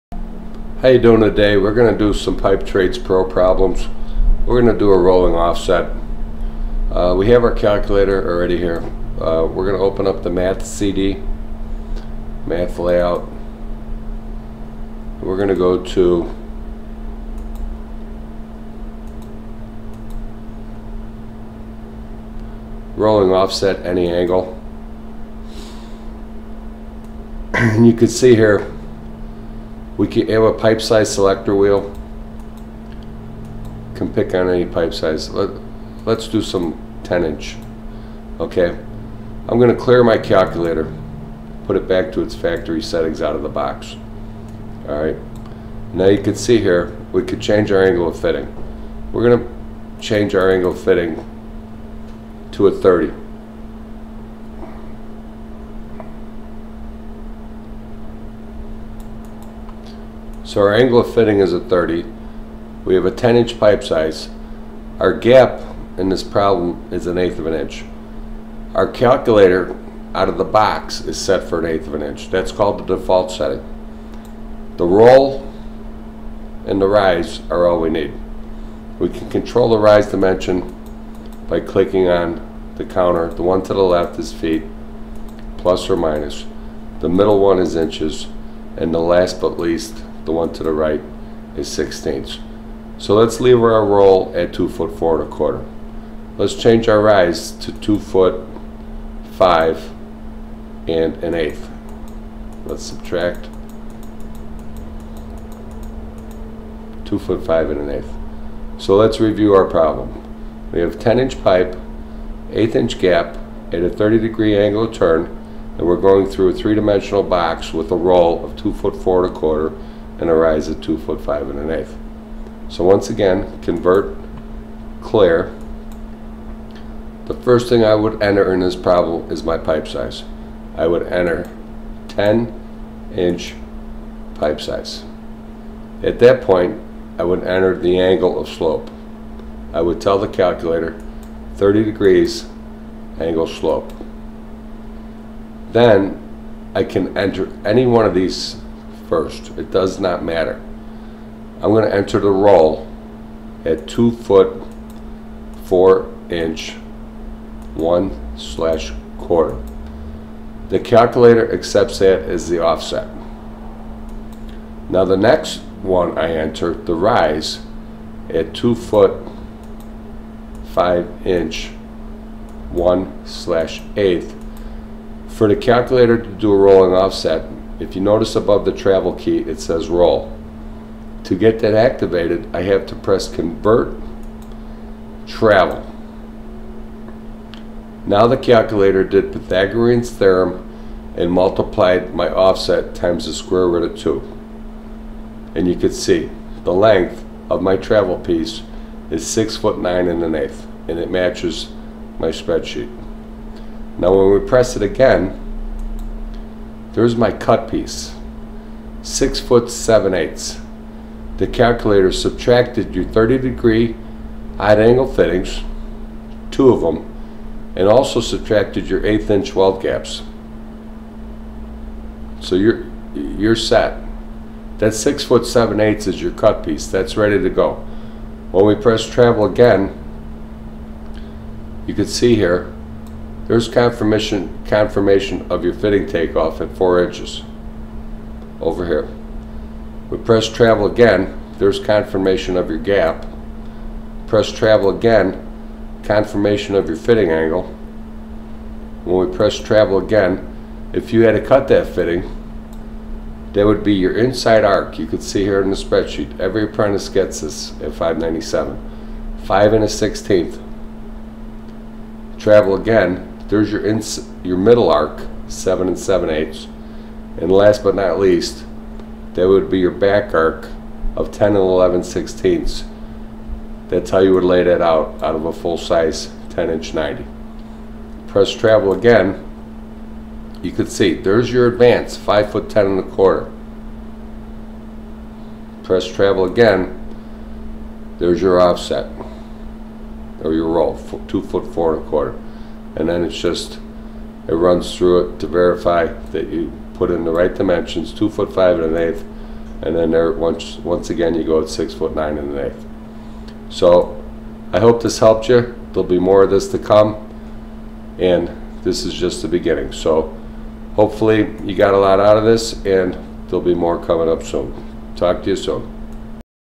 Effortless Rolling Offsets & Weld Fittings Calculations Using Pipetrades Pro Calculator
Rolling offset using Pipe Trades Pro.Moes Math,pipefitting tricks,